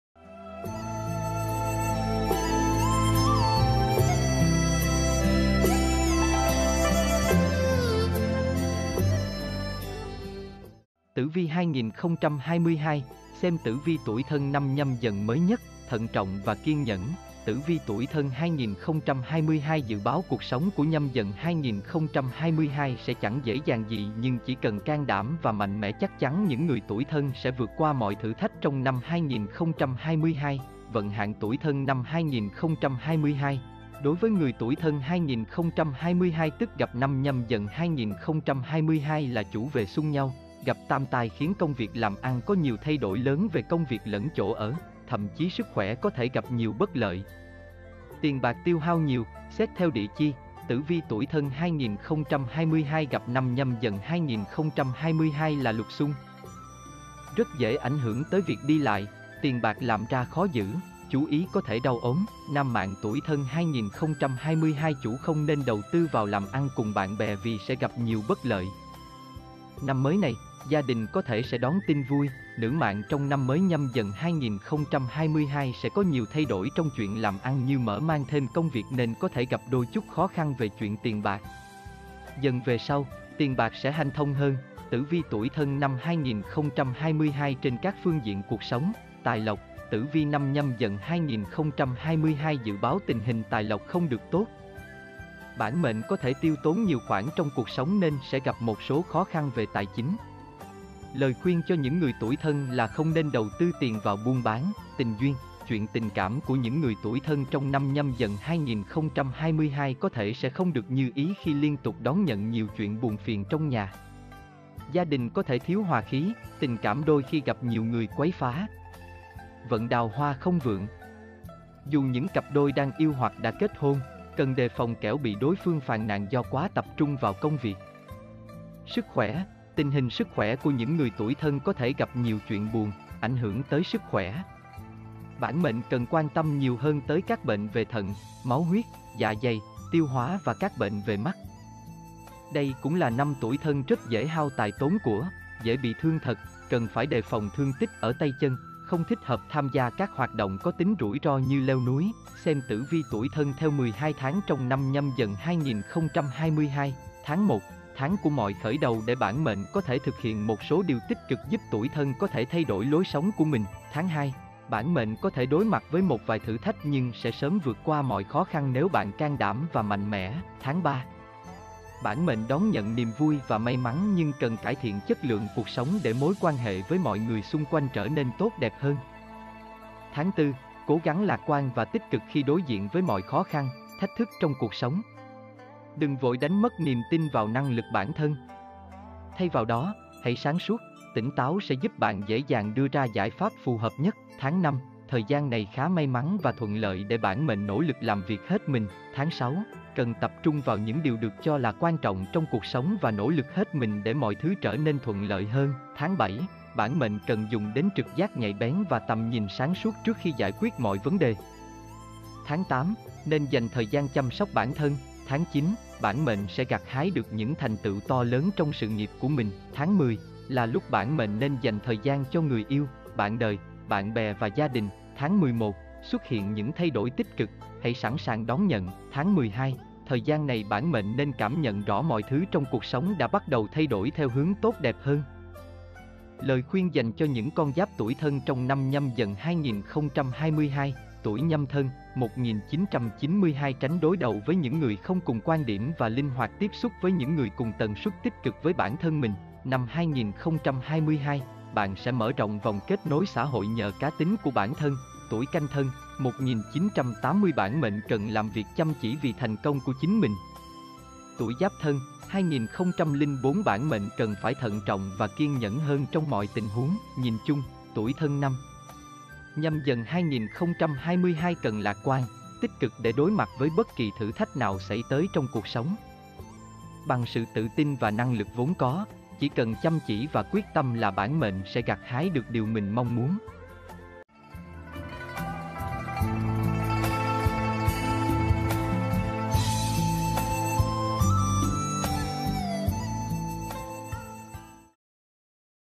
Tử vi 2022, xem tử vi tuổi Thân năm Nhâm Dần mới nhất: Thận trọng và kiên nhẫn
Vận hạn tuổi Thân năm 2022

Đối với người tuổi Thân 2022 tức Gặp năm Nhâm Dần 2022 là chủ về xung nhau, gặp tam tai khiến công việc làm ăn có nhiều thay đổi lớn về công việc lẫn chỗ ở, thậm chí sức khỏe có thể gặp nhiều bất lợi. Tiền bạc tiêu hao nhiều.

Xét theo địa chi, tử vi tuổi Thân 2022 gặp năm Nhâm Dần 2022 là Lục Xung, rất dễ ảnh hưởng tới việc đi lại, tiền bạc làm ra khó giữ, chú ý có thể đau ốm.

Nam mạng tuổi Thân 2022 chủ không nên đầu tư vào làm ăn cùng bạn bè vì sẽ gặp nhiều bất lợi. Năm mới này, gia đình có thể sẽ đón tin vui.

Nữ mạng trong năm mới Nhâm Dần 2022 sẽ có nhiều thay đổi trong chuyện làm ăn như mở mang thêm công việc nên có thể gặp đôi chút khó khăn về chuyện tiền bạc. Dần về sau, tiền bạc sẽ hanh thông hơn.Tử vi tuổi Thân năm 2022 trên các phương diện cuộc sống

Tài lộc

Tử vi năm Nhâm Dần 2022 dự báo tình hình tài lộc không được tốt. Bản mệnh có thể tiêu tốn nhiều khoản trong cuộc sống nên sẽ gặp một số khó khăn về tài chính. Lời khuyên cho những người tuổi Thân là không nên đầu tư tiền vào buôn bán.

Tình duyên

Chuyện tình cảm của những người tuổi Thân trong năm Nhâm Dần 2022 có thể sẽ không được như ý khi liên tục đón nhận nhiều chuyện buồn phiền trong nhà. Gia đình có thể thiếu hoà khí.

Tình cảm đôi khi gặp nhiều người quấy phá. Vận đào hoa không vượng. Dù những cặp đôi đang yêu hoặc đã kết hôn, cần đề phòng kẻo bị đối phương phàn nàn do quá tập trung vào công việc.

Sức khỏe

Tình hình sức khỏe của những người tuổi Thân có thể gặp nhiều chuyện buồn, ảnh hưởng tới sức khỏe. Bản mệnh cần quan tâm nhiều hơn tới các bệnh về thận, máu huyết, dạ dày, tiêu hoá và các bệnh về mắt.

Đây cũng là năm tuổi Thân rất dễ hao tài tốn của, dễ bị thương thật, cần phải đề phòng thương tích ở tay chân, không thích hợp tham gia các hoạt động có tính rủi ro như leo núi,…
Xem tử vi tuổi Thân theo 12 tháng trong năm Nhâm Dần 2022

Tháng 1: Tháng của mọi khởi đầu để bản mệnh có thể thực hiện một số điều tích cực giúp tuổi Thân có thể thay đổi lối sống của mình.

Tháng 2: Bản mệnh có thể đối mặt với một vài thử thách nhưng sẽ sớm vượt qua mọi khó khăn nếu bạn can đảm và mạnh mẽ.

Tháng 3: Bản mệnh đón nhận niềm vui và may mắn nhưng cần cải thiện chất lượng cuộc sống để mối quan hệ với mọi người xung quanh trở nên tốt đẹp hơn.

Tháng 4: Cố gắng lạc quan và tích cực khi đối diện với mọi khó khăn, thách thức trong cuộc sống. Đừng vội đánh mất niềm tin vào năng lực bản thân. Thay vào đó, hãy sáng suốt, tỉnh táo sẽ giúp bạn dễ dàng đưa ra giải pháp phù hợp nhất.

Tháng 5: Thời gian này khá may mắn và thuận lợi để bản mệnh nỗ lực làm việc hết mình.

Tháng 6: Cần tập trung vào những điều được cho là quan trọng trong cuộc sống và nỗ lực hết mình để mọi thứ trở nên thuận lợi hơn.

Tháng 7: Bản mệnh cần dùng đến trực giác nhạy bén và tầm nhìn sáng suốt trước khi giải quyết mọi vấn đề.

Tháng 8: Nên dành thời gian chăm sóc bản thân.

Tháng 9: Bản mệnh sẽ gặt hái được những thành tựu to lớn trong sự nghiệp của mình.

Tháng 10: Là lúc bản mệnh nên dành thời gian cho người yêu, bạn đời, bạn bè và gia đình.

Tháng 11: Xuất hiện những thay đổi tích cực, hãy sẵn sàng đón nhận.

Tháng 12: Thời gian này bản mệnh nên cảm nhận rõ mọi thứ trong cuộc sống đã bắt đầu thay đổi theo hướng tốt đẹp hơn.
Lời khuyên dành cho những con giáp tuổi Thân trong năm Nhâm Dần 2022

Tuổi Nhâm Thân (1992): Tránh đối đầu với những người không cùng quan điểm và linh hoạt tiếp xúc với những người cùng tần suất tích cực với bản thân mình.

Năm 2022, bạn sẽ mở rộng vòng kết nối xã hội nhờ cá tính của bản thân.

Tuổi Canh Thân (1980): Bản mệnh cần làm việc chăm chỉ vì thành công của chính mình.

Tuổi Giáp Thân (2004): Bản mệnh cần phải thận trọng và kiên nhẫn hơn trong mọi tình huống.

Nhìn chung, tuổi Thân năm Nhâm Dần 2022 cần lạc quan, tích cực để đối mặt với bất kỳ thử thách nào xảy tới trong cuộc sống. Bằng sự tự tin và năng lực vốn có, chỉ cần chăm chỉ và quyết tâm là bản mệnh sẽ gặt hái được điều mình mong muốn.